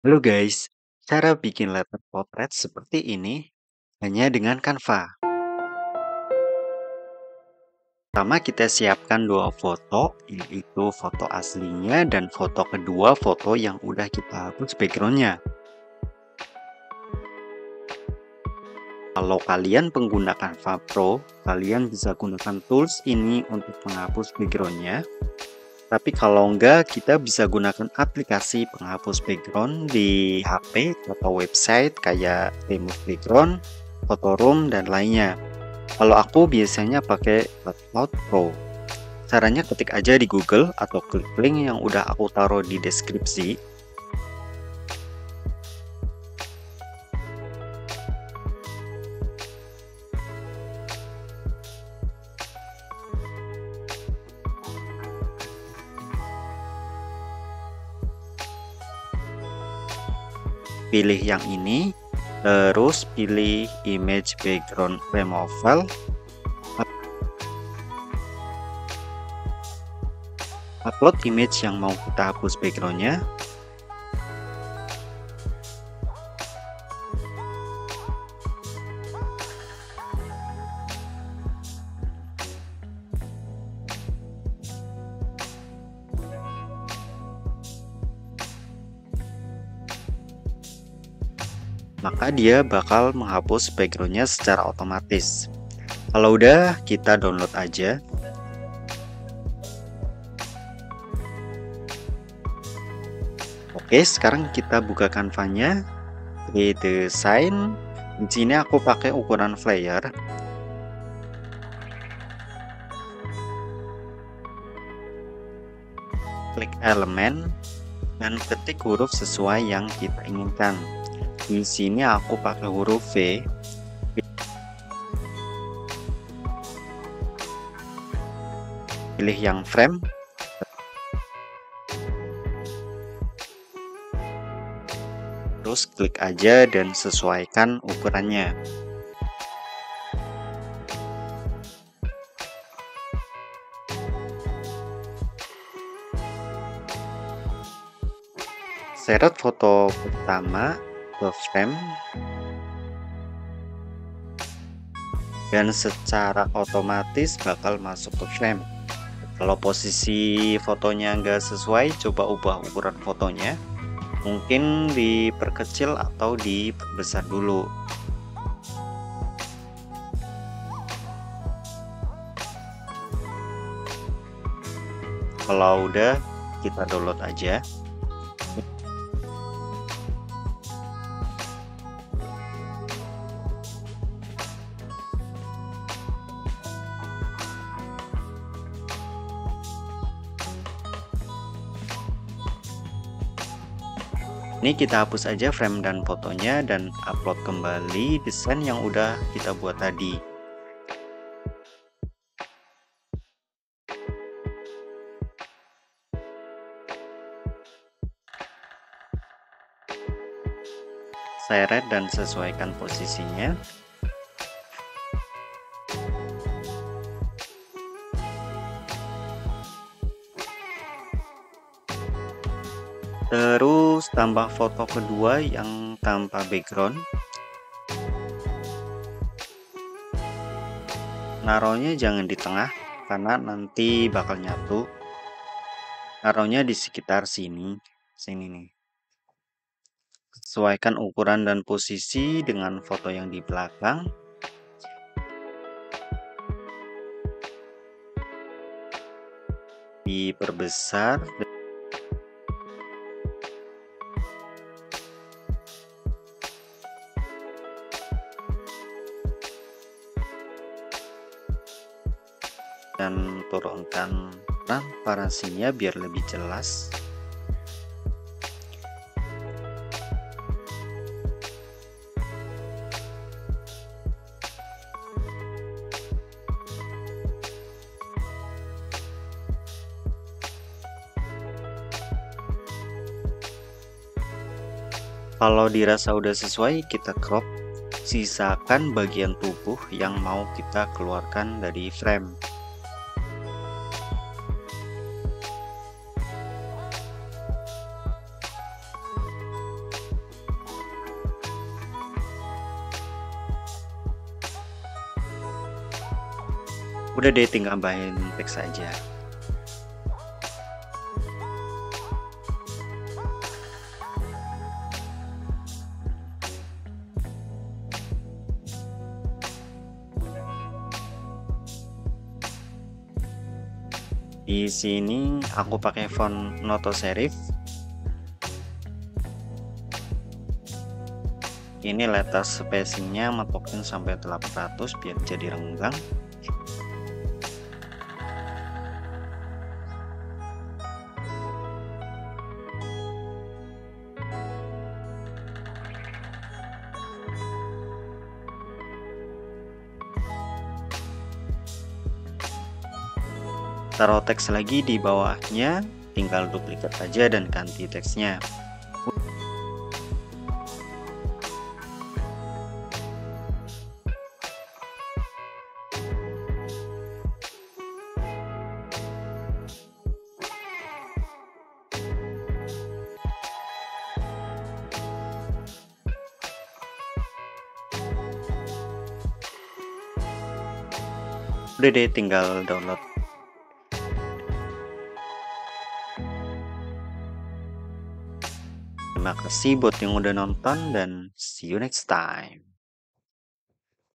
CARA MEMBUAT LETTER PORTRAIT DI CANVA
Hello guys di video ini aku akan membagikan cara membuat letter portrait di canva tanpa meggunakan photoshop. Jika ada bagian yang kurang jelas silahkan tanyakan saja di kolom komentar.